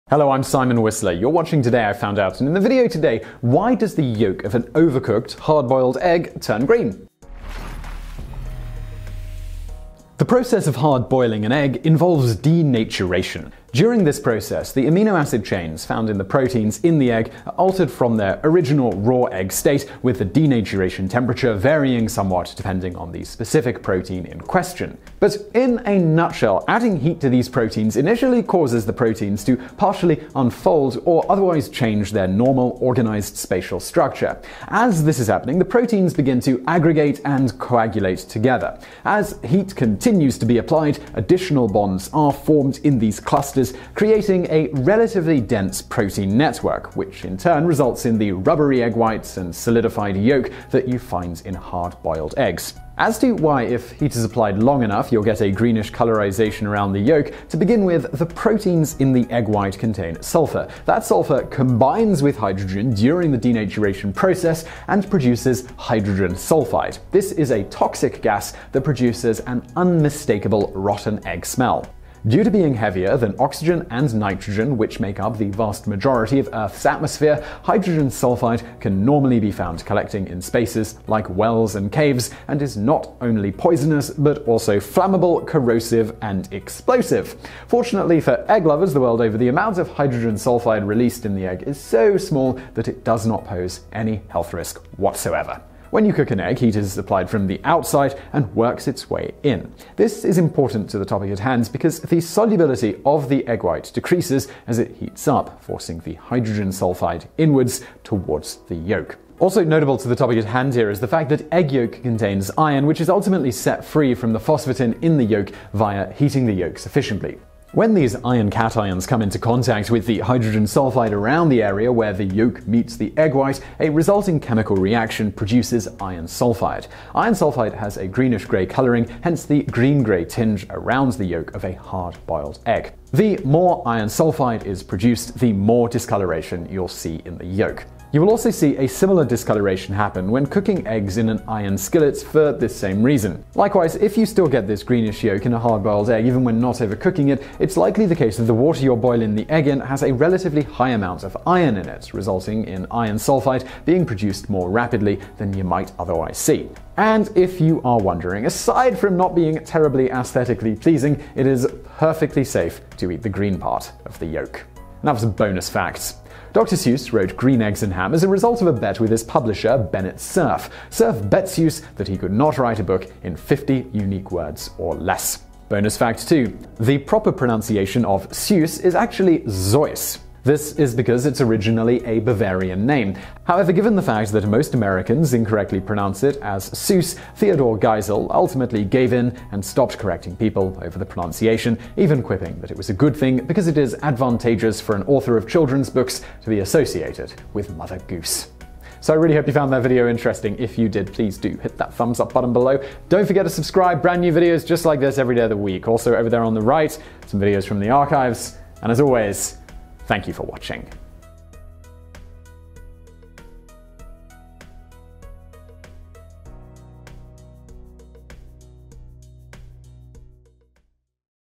Why Does the Yolk of an Overcooked Hard Boiled Egg Turn Green
In this video:

The process of hard-boiling an egg involves denaturation. During this process, the amino acid chains found in the proteins in the egg are altered from their original, raw egg state, with the denaturation temperature varying somewhat depending on the specific protein in question (see chart here). But in a nutshell, adding heat to these proteins initially causes the proteins to partially unfold or otherwise change their normal organized spatial structure. As this is happening, the proteins begin to aggregate and coagulate together. As heat continues to be applied, additional bonds are formed in these clusters, creating a relatively dense protein network, which in turn results in the rubbery egg whites and solidified yolk you find in hard-boiled eggs.